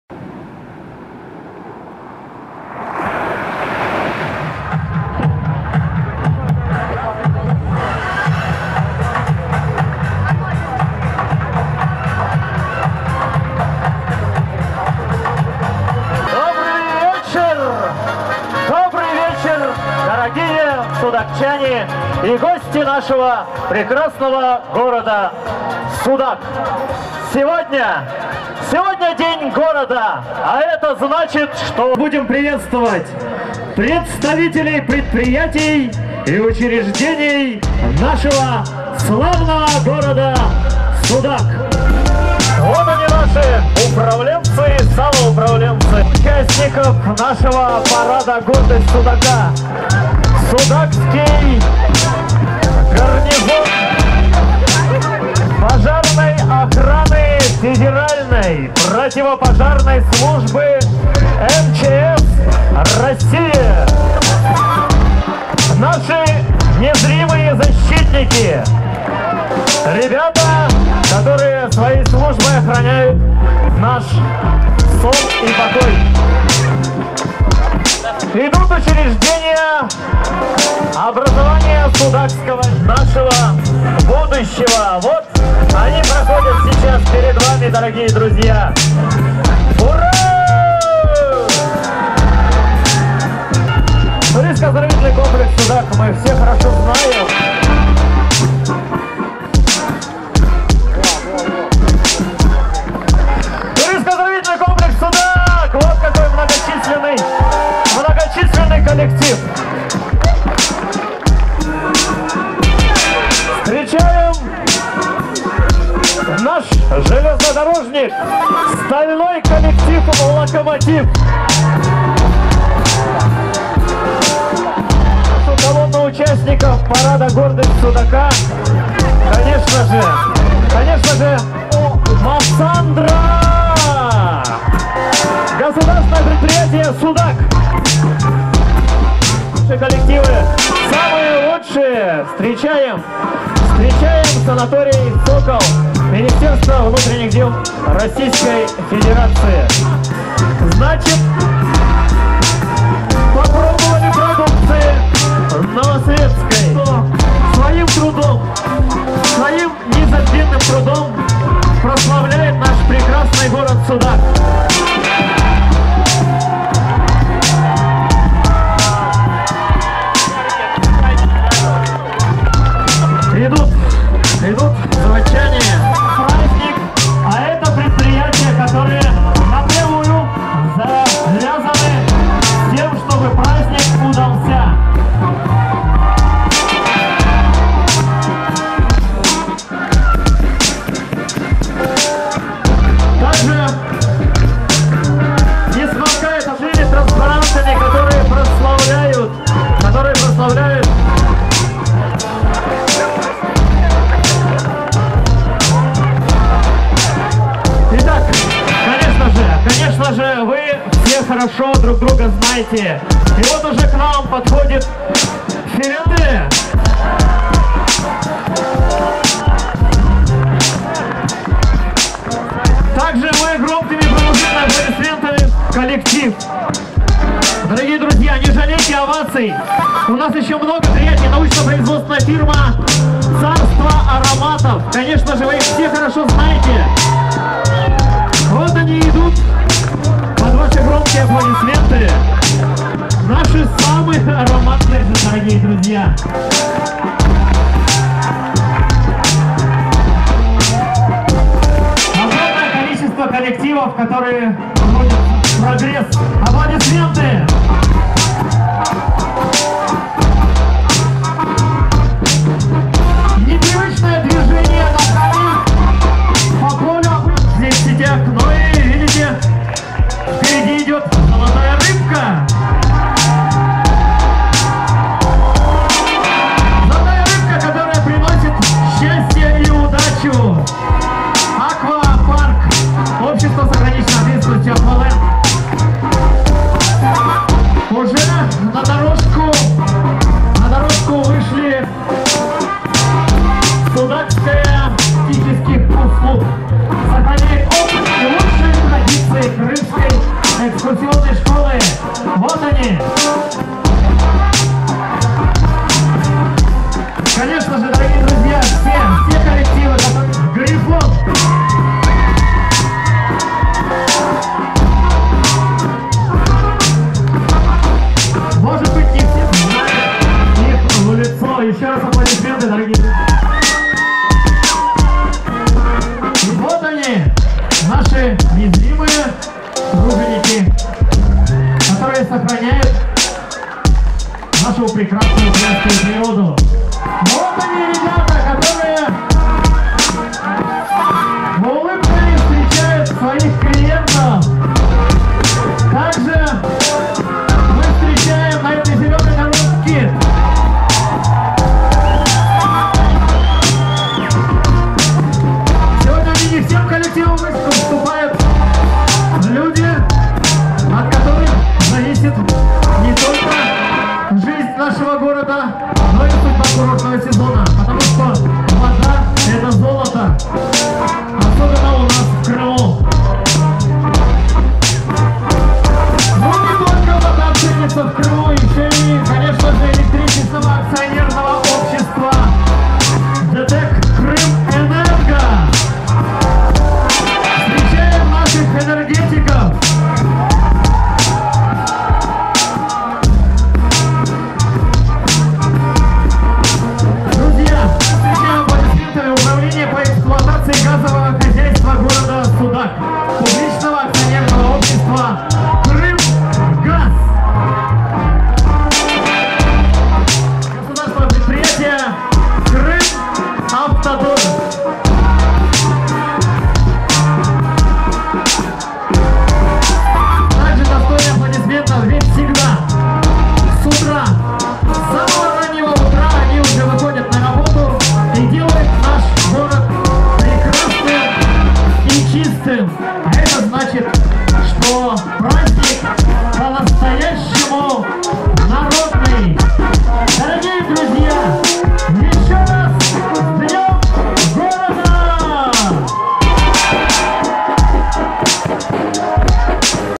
Парад коллективов в Судаке
12 сентября 2014 года Судак встретил свой день рождения первый раз в составе Российской Федерации.
В анонсах города был объявлен музыкальный сюрприз для судакчан. И им стала легендарная группа 80-ых Secret Service. А сейчас взгляните на Парад коллективов города.